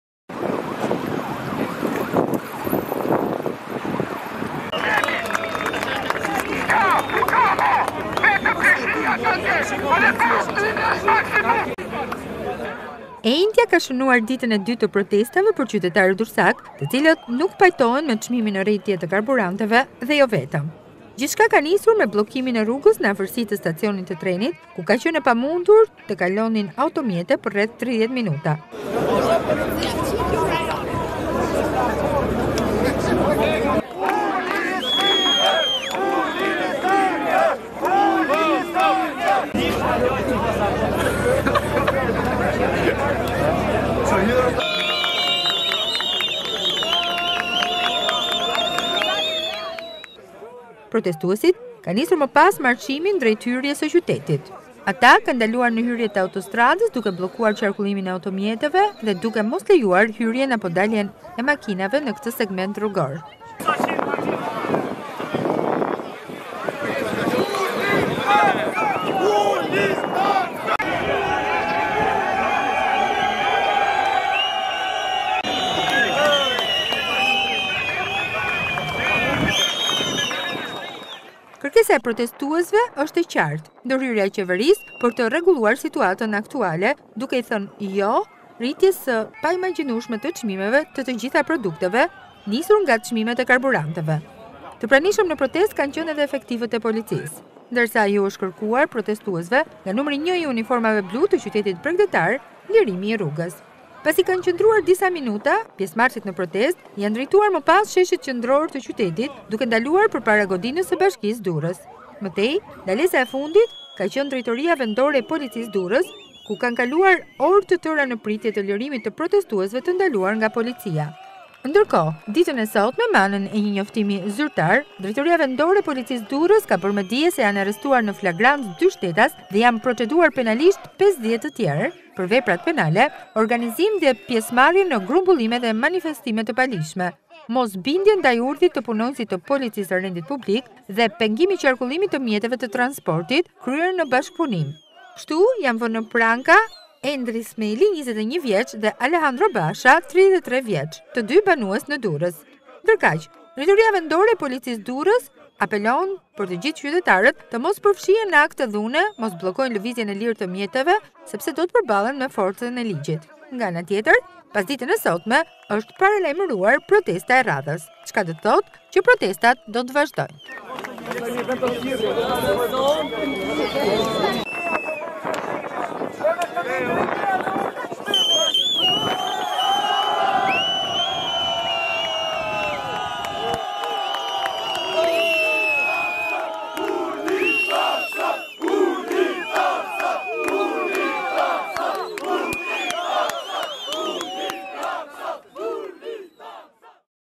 Protestat nuk ndalen në Durrës, në pranga dy të rinj akuzohen për tri vepra penale, 50 të proçeduar
Protestat nuk ndalen në Durrës, në pranga dy të rinj akuzohen për tri vepra penale, 50 të proçeduar nga policia